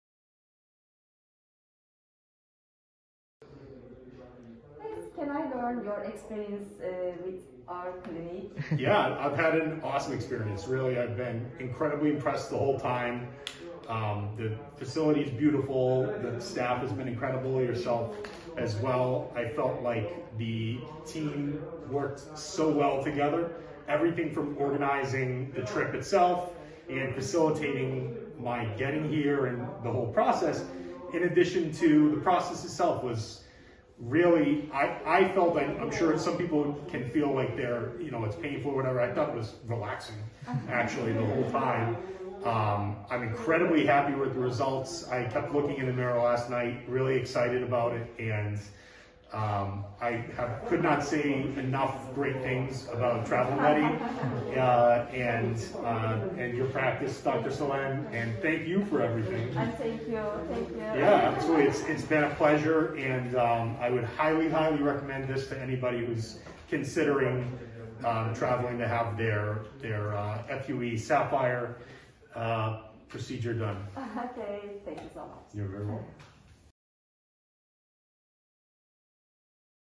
Пересадка волосся в Туреччині - Відгук від Джоуі Ф. - Після пересадки волосся
Джоуі Ф. обрав доктора Ойку Челен / TravelMEDI для пересадки волосся

Зв'яжіться з нами для операції пересадки волосся чи наших інших процедур